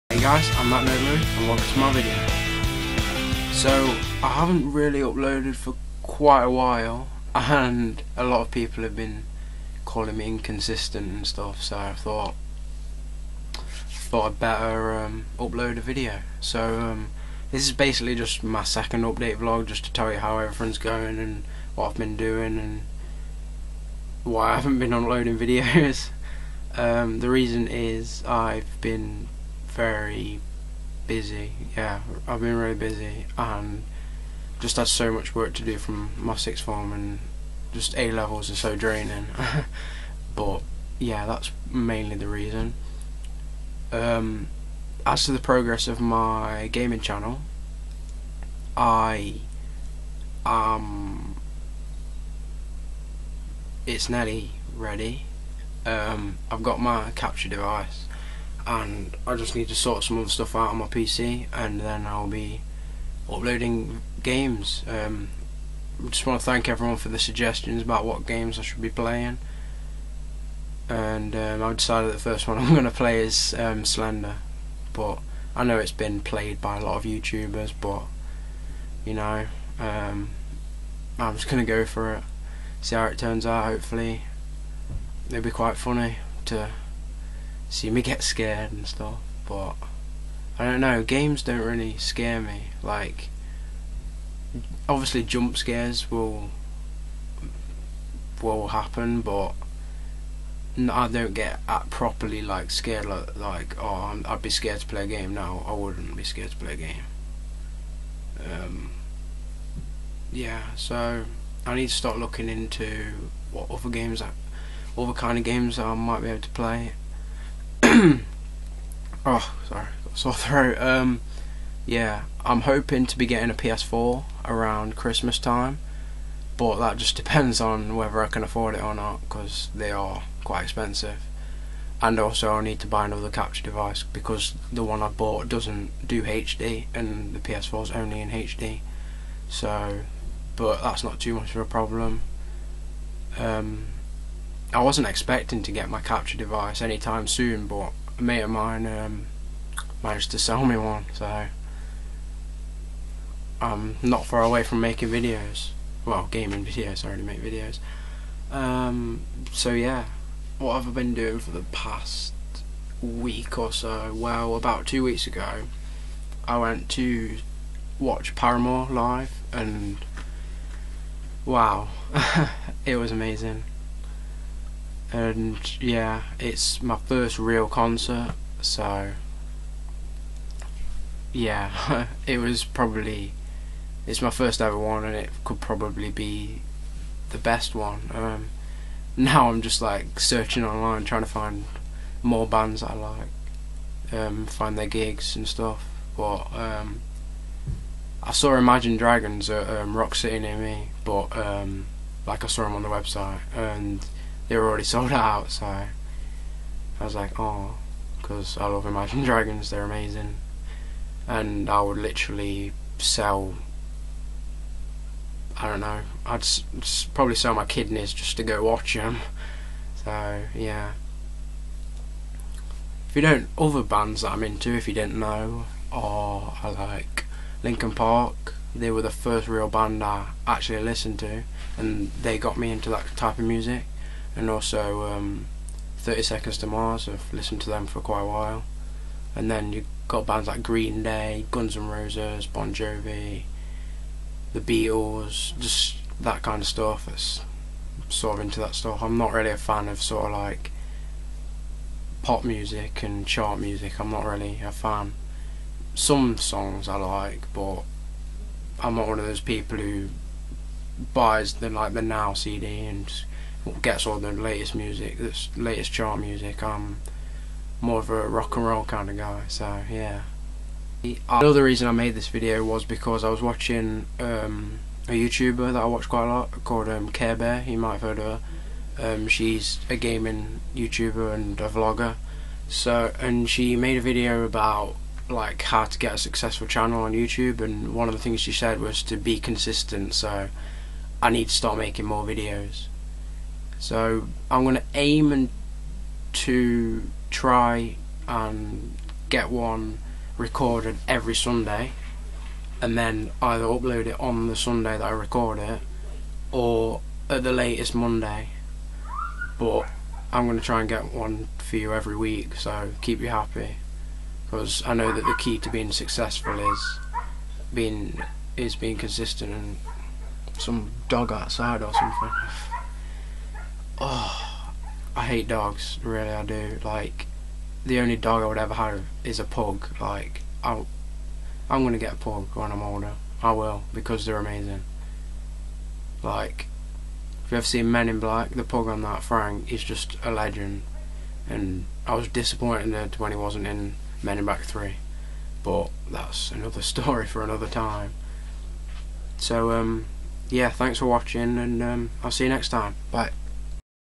Update Vlog #2
Haven't uploaded for a while so heres me second update vlog while i prepare my gaming videos, hope you enjoy!